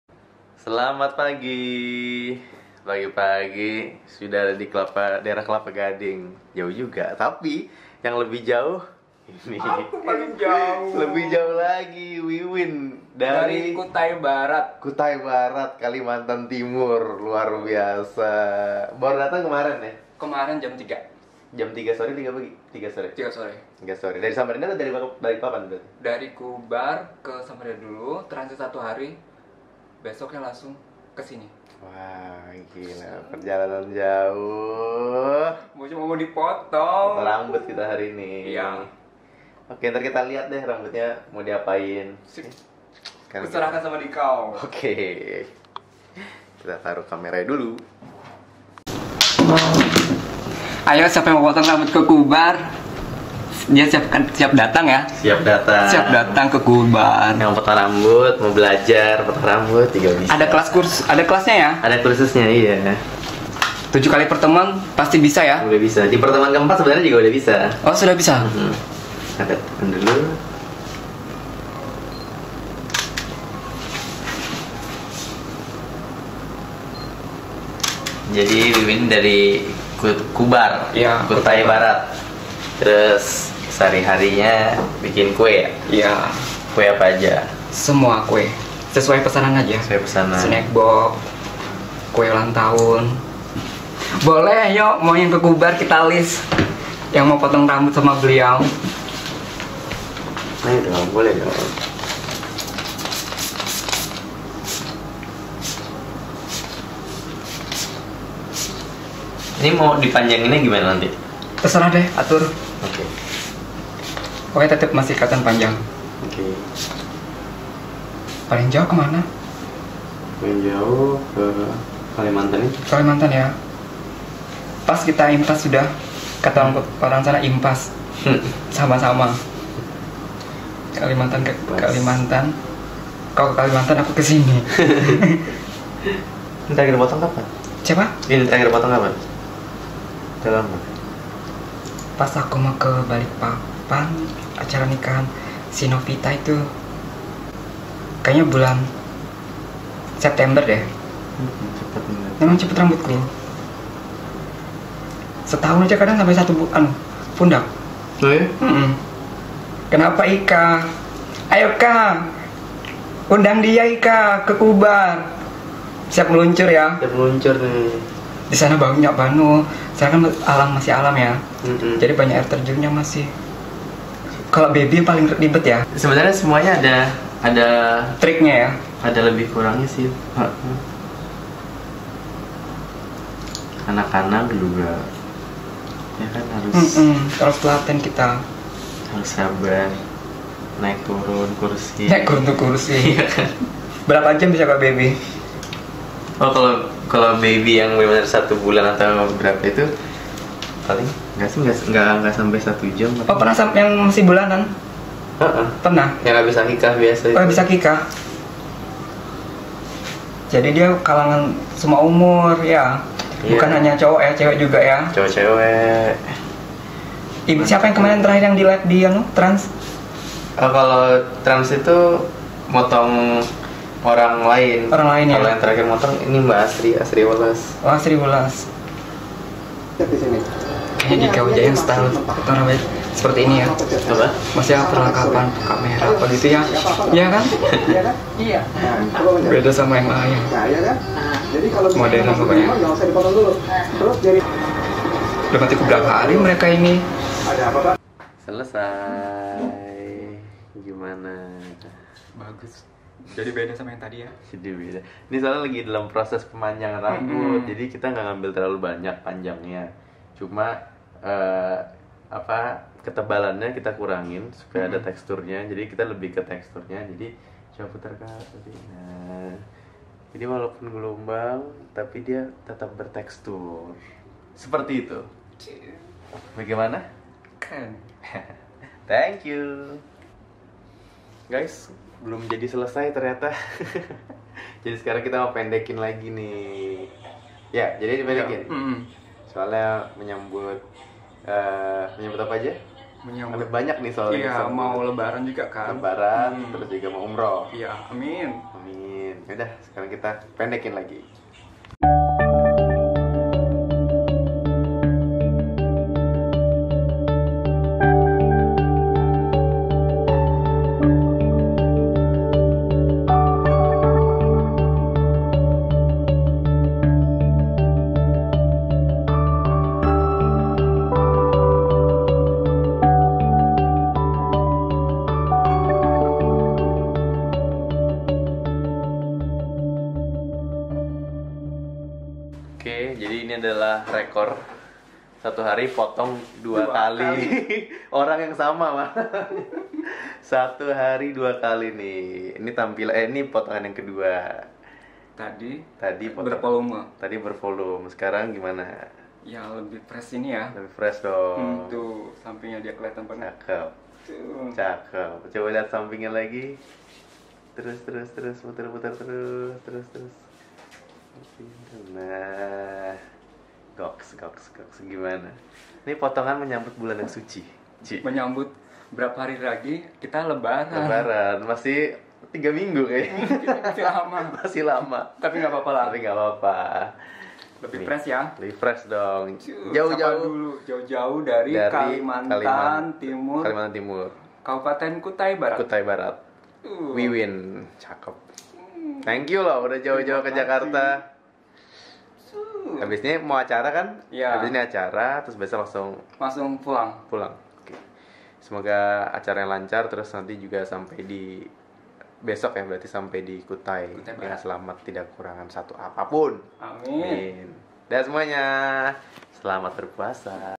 Client dari Kutai Barat
Percaya enggak gaes ada Client yang datang jauh-jauh dari Kutai Barat ke Jakarta untuk potong rambut?!
Percaya enggak percaya, beneran ada, namanya Wiwin, dari Kutai Barat. Yuk tonton video selengkapnya, selamat menikmati :)